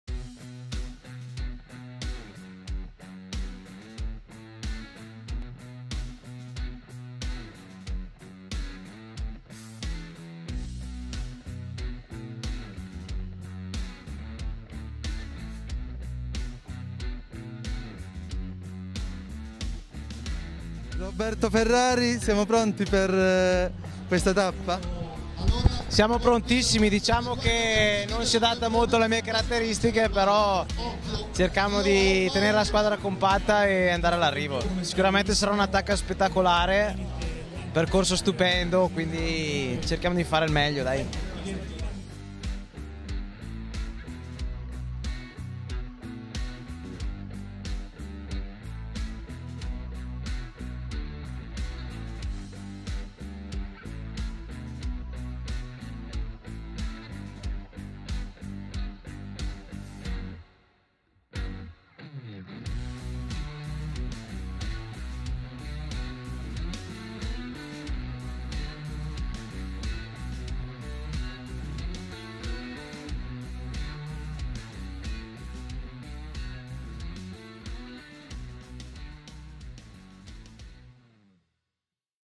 Giro E 2021 - Intervista a Roberto Ferrari - Tappa 9
Giro E 2021 - Intervista a Roberto Ferrari- Tappa 9 - (Sulmona - Campo Felice Rocca di Cambio) - 16 maggio 2021- A cura di Antonio Abbruzzese, Giuseppe Girolamo, Filippo Mazzullo, Riccardo Cristiano, Gianni Colacione.